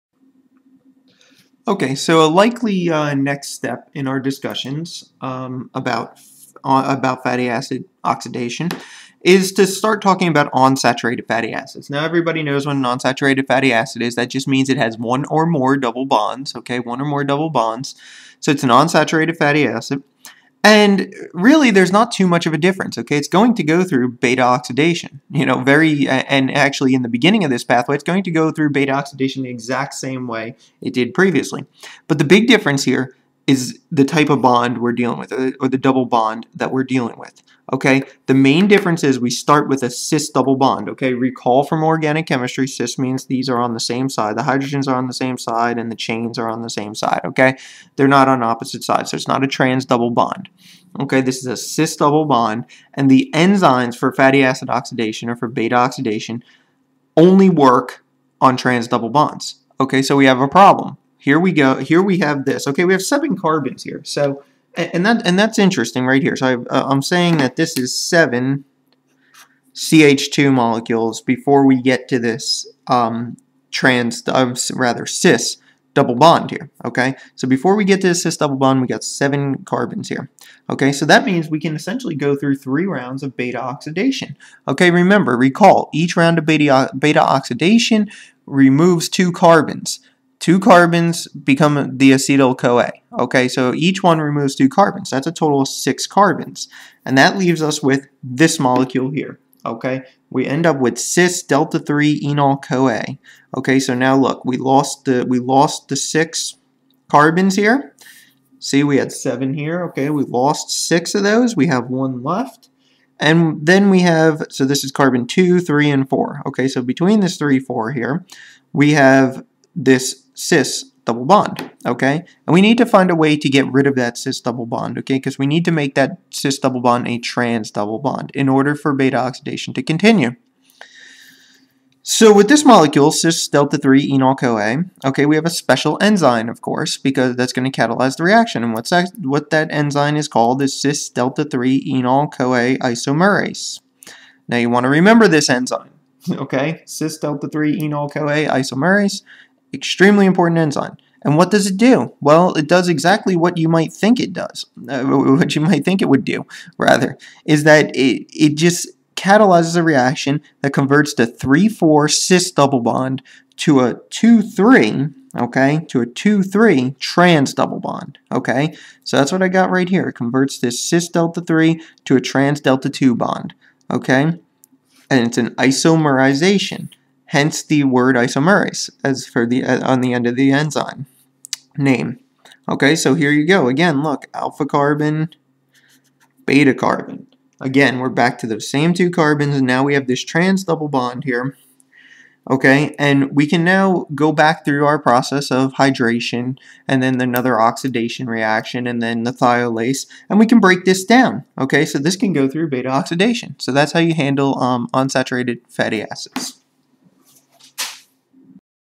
B-oxidation of unsaturated Fatty acids
This video provides the details for B-oxidation of unsaturated fatty acids which has some important differences that need to be understood.